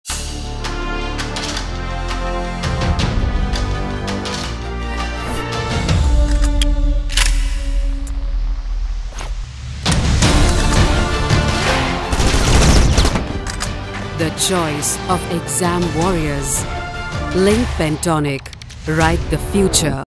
Linc Pens | Pentonic - Exam Warriors 20 sec
Be exam ready with Pentonic, the writing-weapon that never lets you down. Face the upcoming examinations like a warrior without fear.